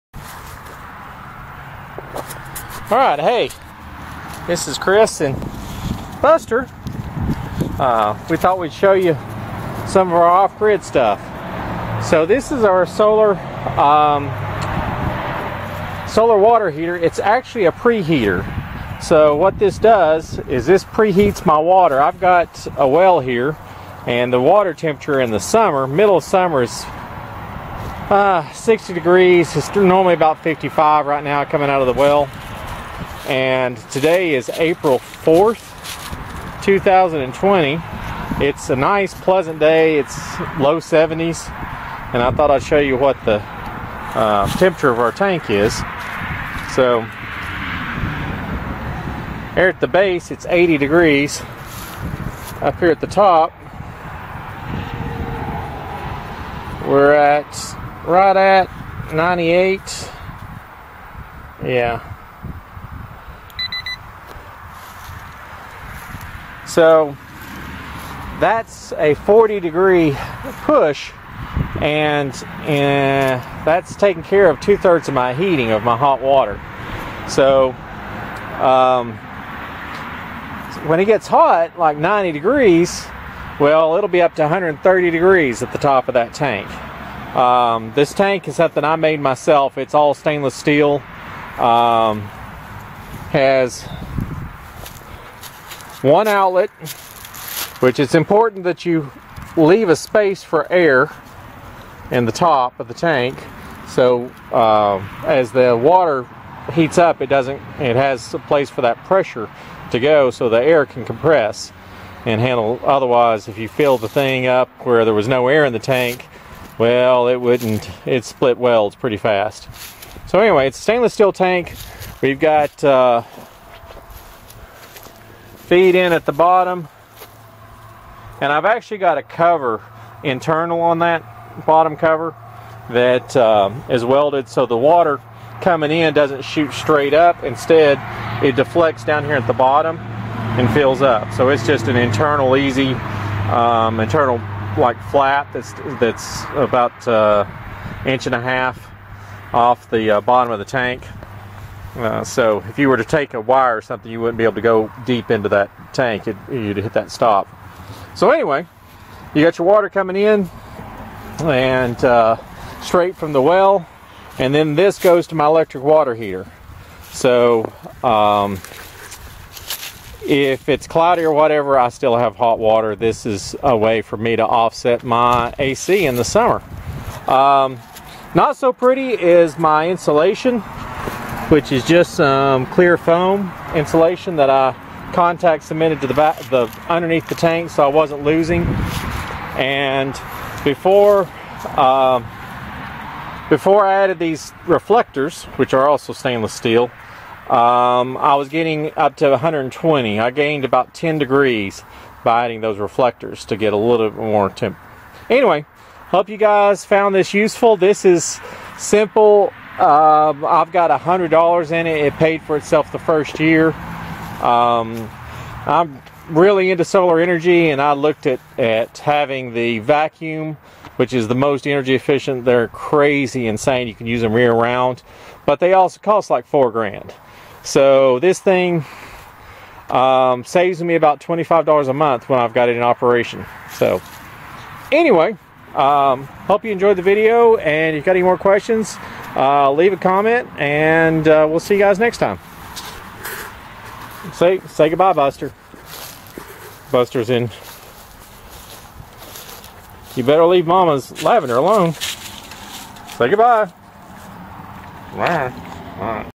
basic and cheap solar water heater
a quick look at my solar water preheater. it is basic unit but paid for itself the first year i used it. I have it in operation for 7 months of the year providing 60 to 100 percent of my hot water need depending on the month